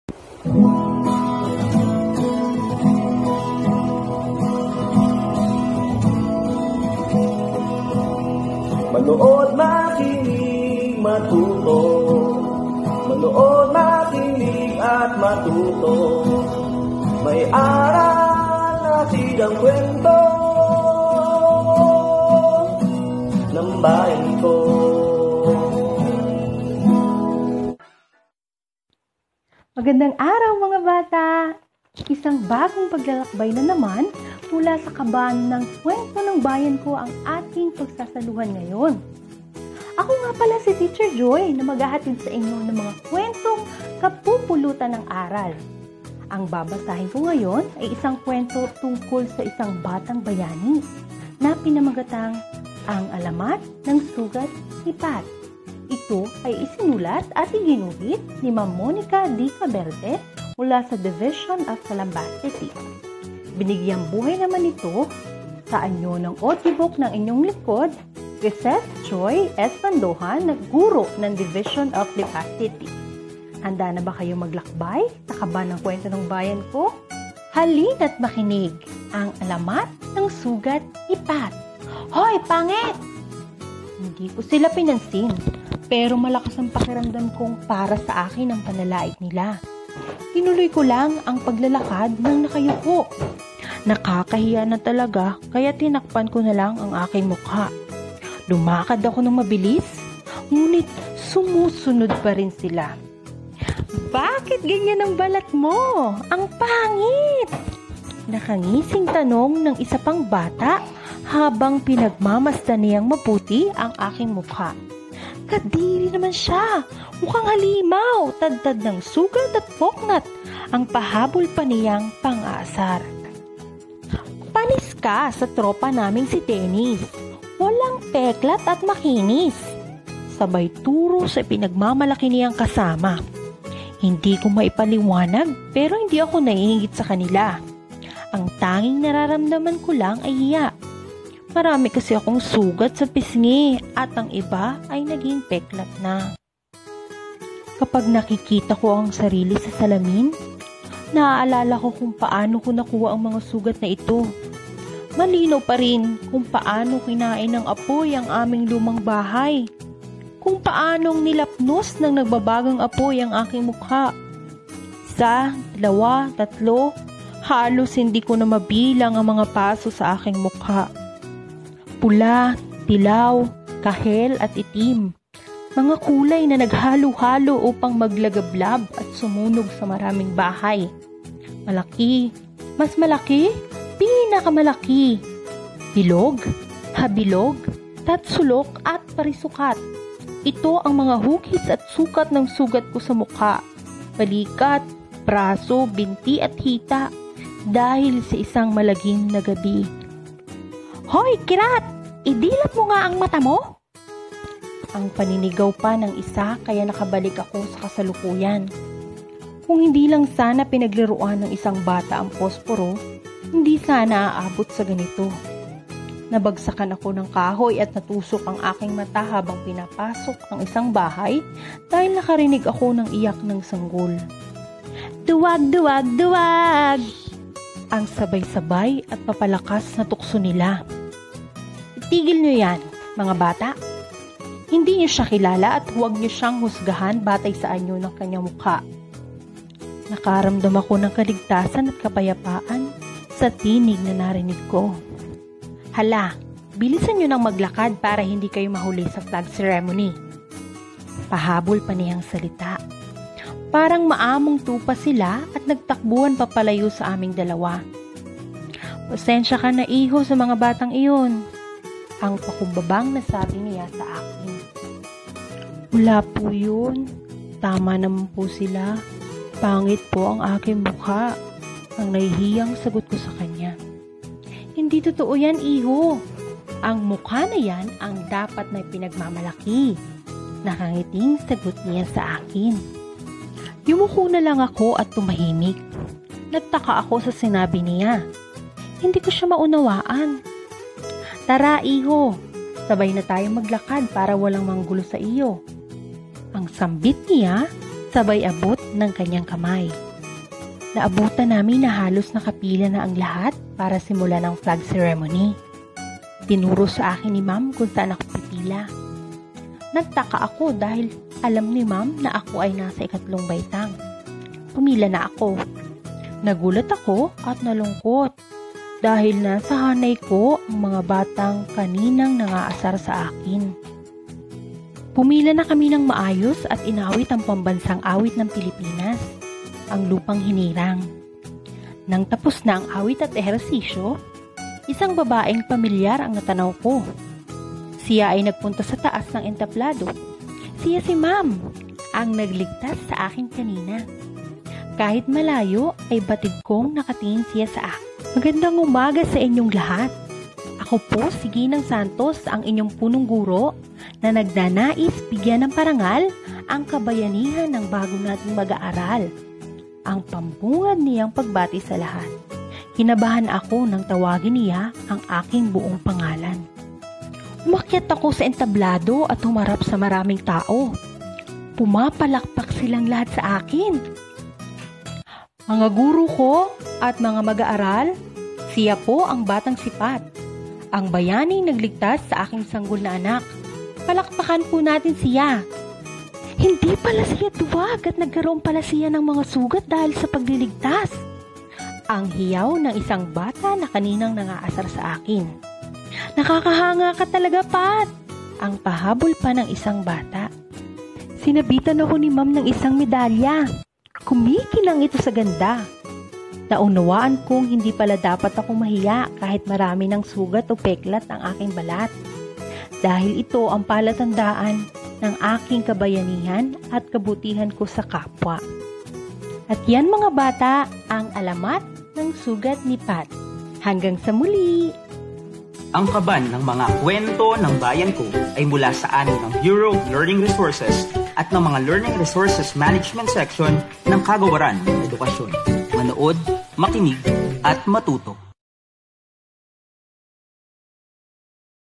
"Ang Alamat ng Sugat ni Pat " Entry #154 ( Kwentong Pambata - DepEd Storybooks )
Audiobook Narration by Cressette Joy S. Bandohan of Lipa City Division

"Ang Alamat ng Sugat ni Pat"

Kuwento at guhit ni Monica D. Caberte Divison of Calamba City

 Kwentong Pambata | Mga Kwentong Pambata | Kwentong Pambata Tagalog | Kwentong Pang Bata | Pambatang Kwento | 4K UHD | Filipino Fairy Tales